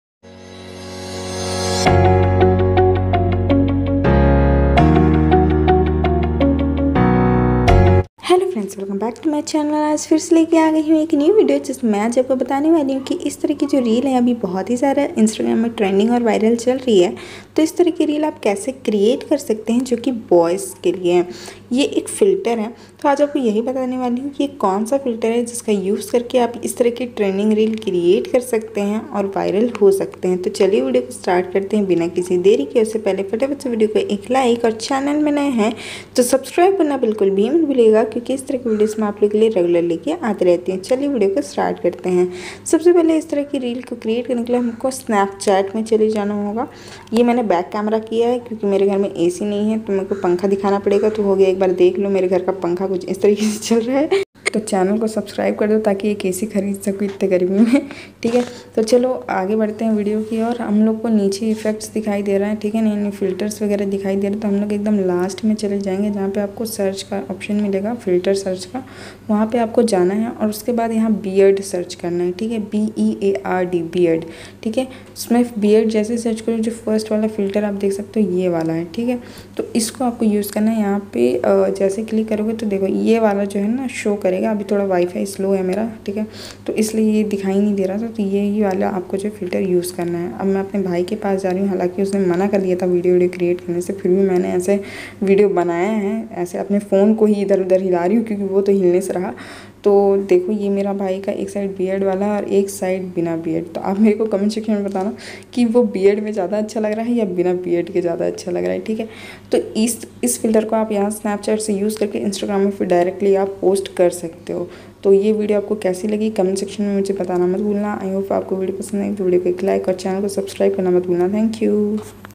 Trending Beard filter reel editing | beardless trending filter | No beard filter
Hello friends aaj mea apko btane ja rhi hu kis trh se ap beard vs no beard look challenge filter use kr k reel create kr skte hea .